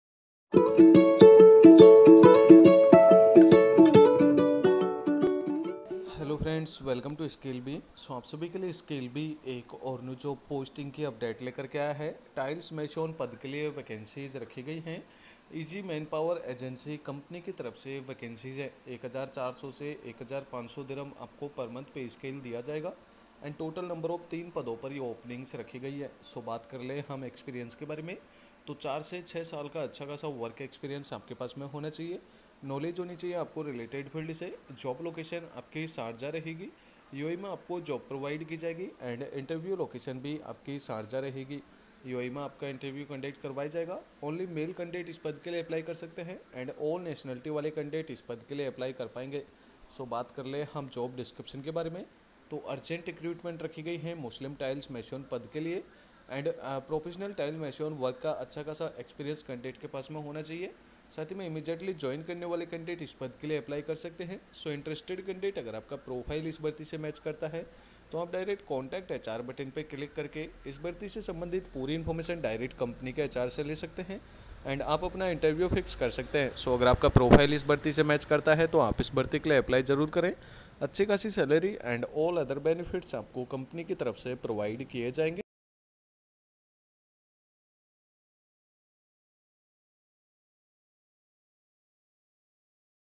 JOB IN UAE / Role: Tiles Mason / Company: Easy Manpower Agency / Salary: AED 1400 - 1500
13...Check out this job opening on the Skillbee app that is available for you:

Role: Tiles Mason

Company: Easy Manpower Agency

Openings: 3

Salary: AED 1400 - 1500

Experience: 4 - 6 years

Job Location: Sharjah - United Arab Emirates

Interview Location: Sharjah - United Arab Emirates